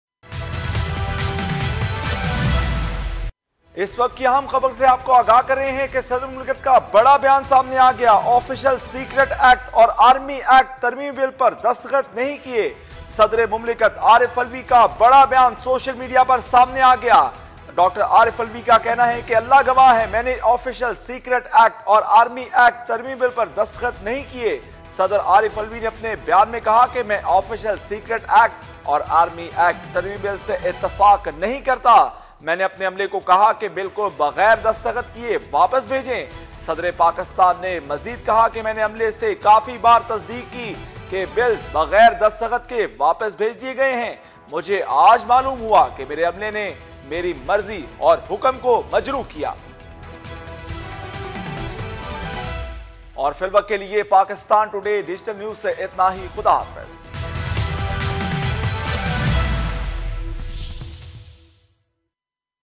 آفیشل سیکریٹ ایکٹ اور آرمی ایکٹ ترمیمی بل صدر مملکت عارف علوی سے کس نے غداری کی ؟| Pakistan Today
Official Secrets Act and Army Act Amendment Bill Who betrayed President Arif Alvi?